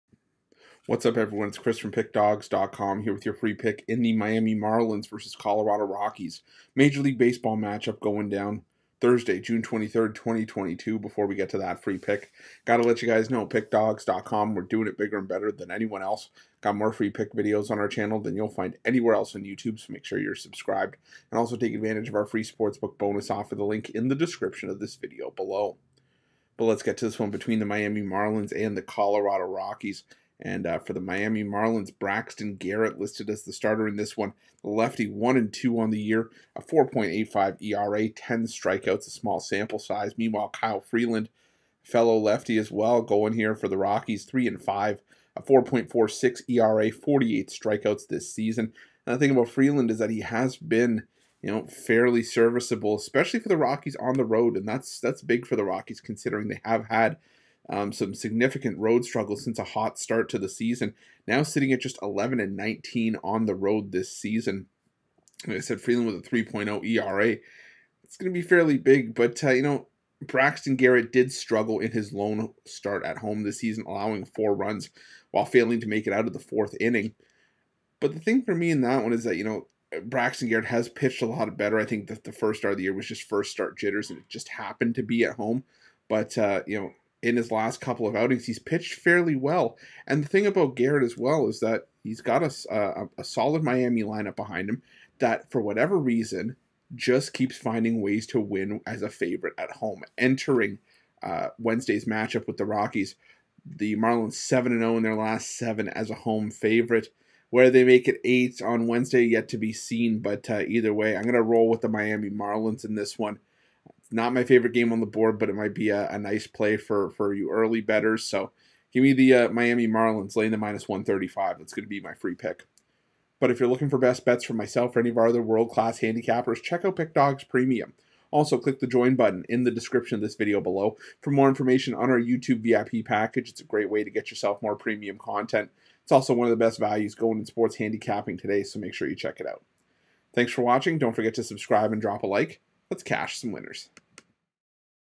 Miami Marlins vs Colorado Rockies 6/23/22 MLB Free Pick Free MLB Betting Tips
The Colorado Rockies face off with the Miami Marlins in Thursday's MLB Baseball action. Get Chris' Expert Pick and Prediction for this game.